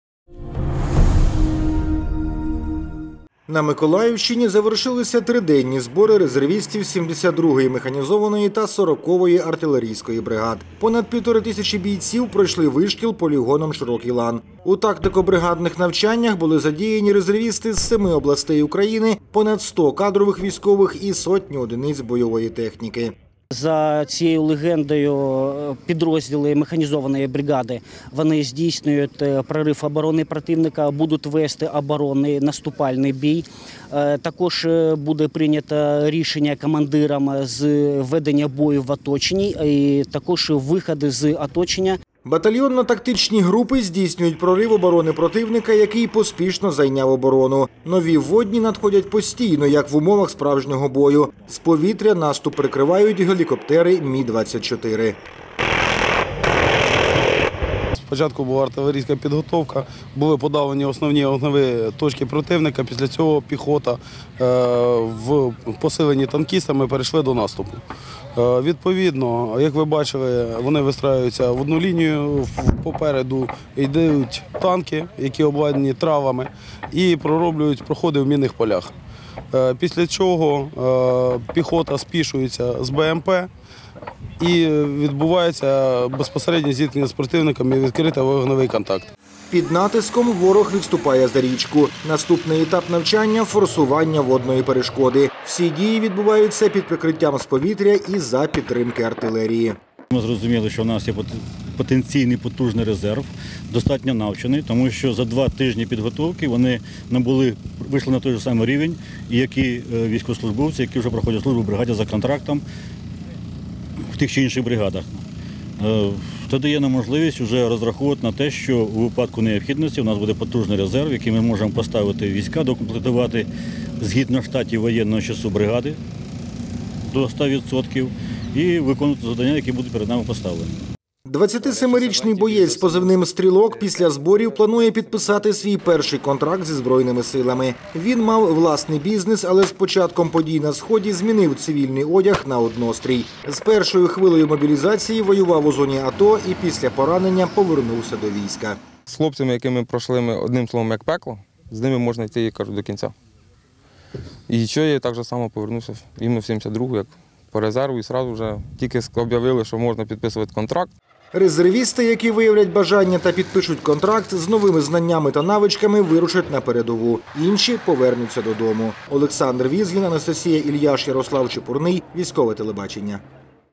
Резервісти 72-ї ОМБр тренувалися на Миколаївщині
На полігоні "Широкий Лан" резервісти 72-ї окремої механізованої бригади тренувалися долати оборону противника під прикриттям армійської авіації та за підтримки артилеристів 40-ї артбригади